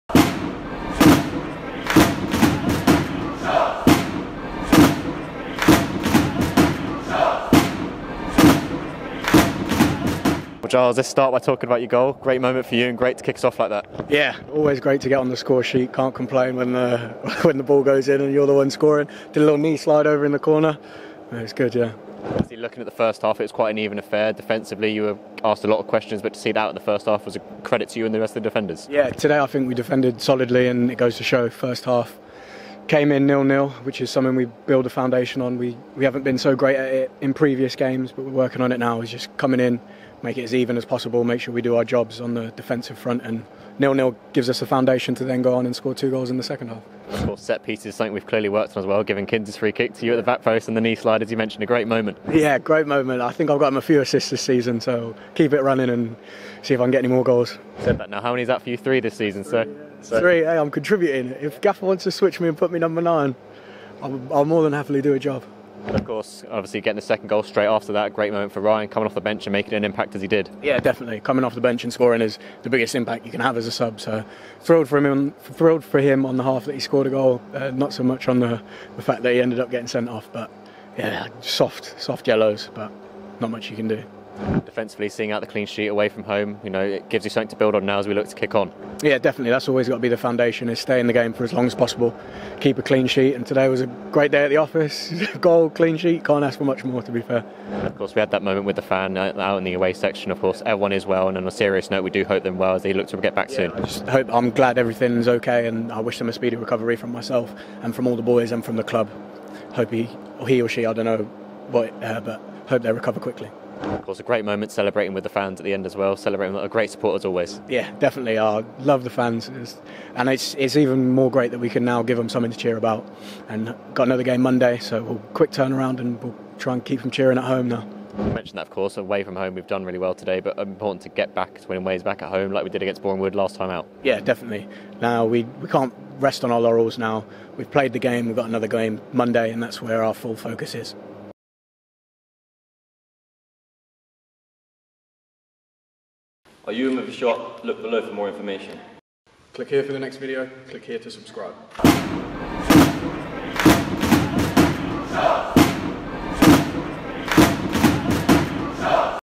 Giles Phillips Post-Match: Yeovil Town (A)
Giles Phillips spoke after our 2-0 away victory against Yeovil Town on Good Friday